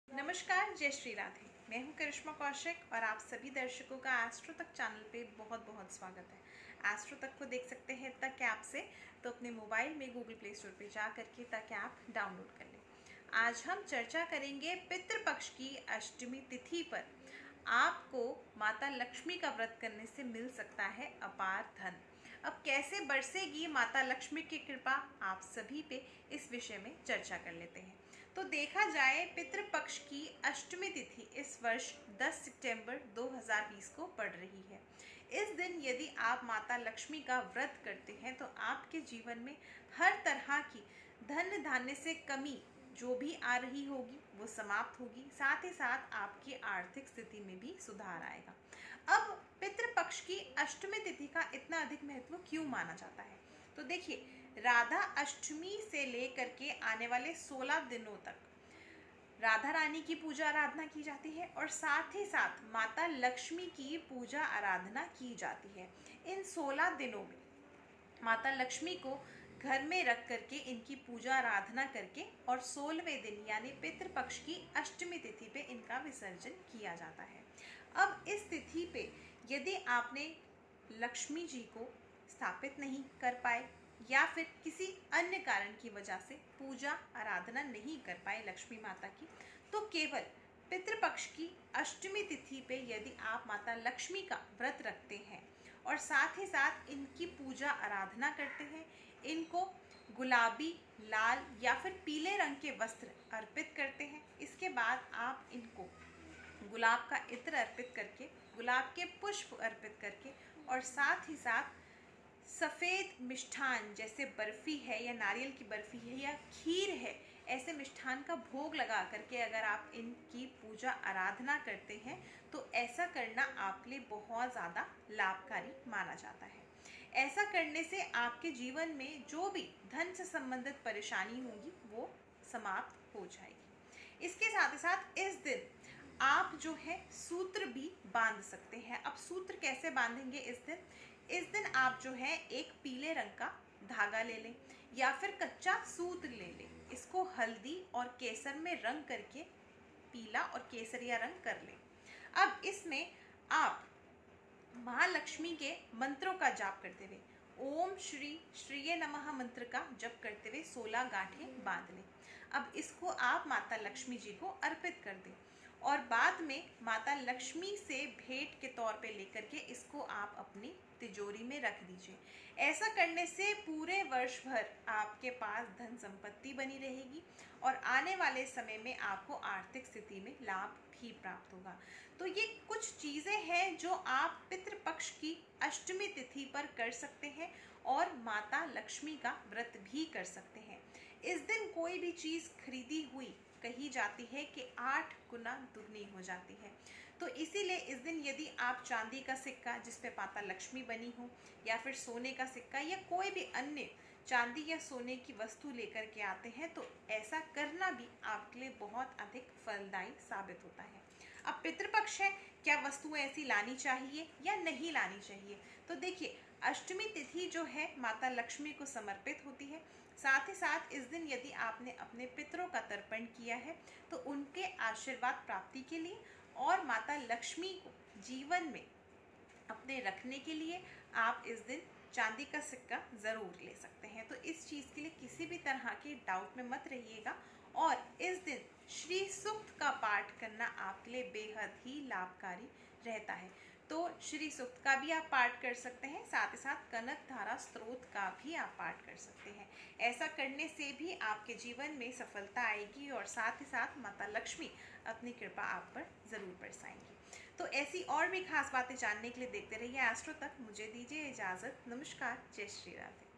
पितृपक्ष की अष्टमी तिथि पर माता लक्ष्मी जी के व्रत से मिलेगा अपार धन | Astro Tak
ज्योतिर्विद करिश्मा कौशिक से जानिए पितृपक्ष की अष्टमी तिथि पर माता लक्ष्मी जी के व्रत से मिलेगा अपार धन... Pitru_Paksha_2020 Karishma_Kaushik AstroTak, (Ask a Question)